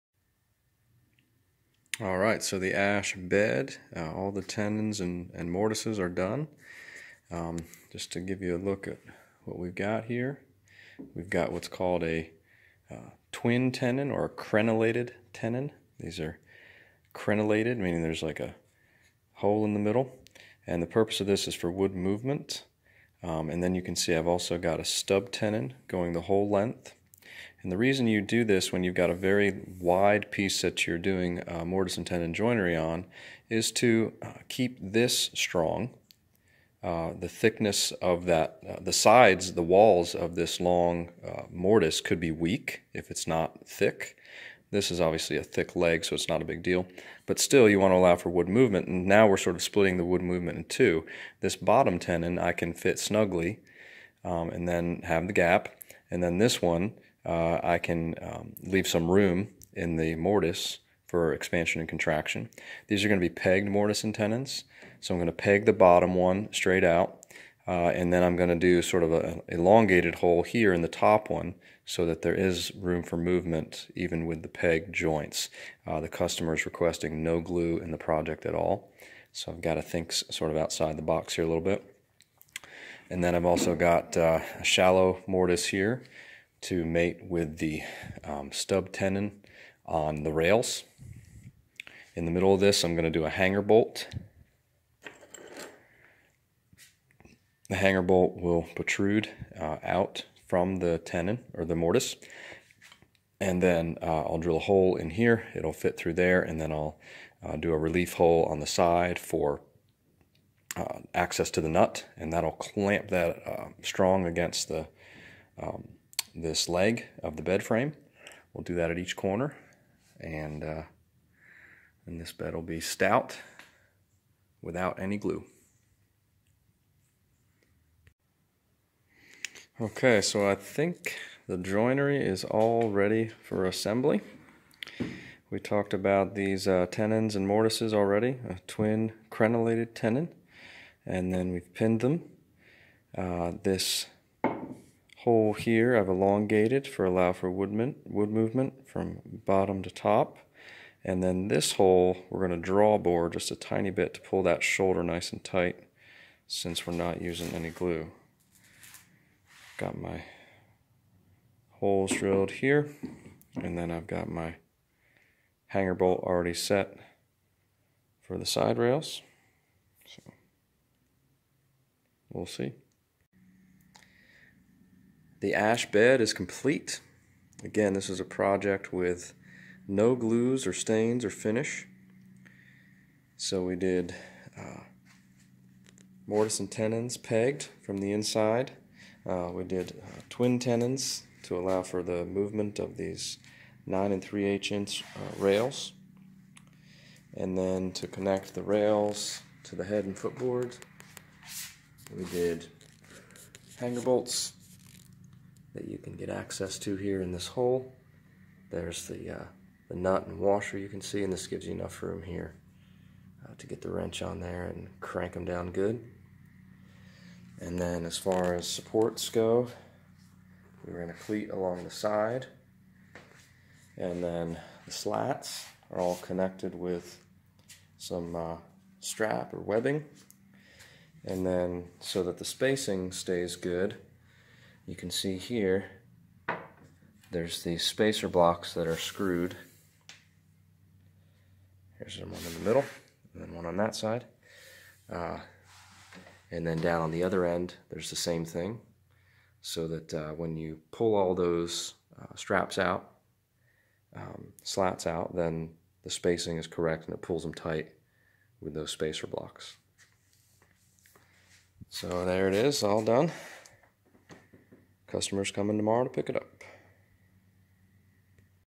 Custom Project - Ash Bed - Mortise and Tenon Bed Frame
Mortise and tenon bed, made of ash. Drawbored mortise and tenons, with no glue. Enjoy the channel?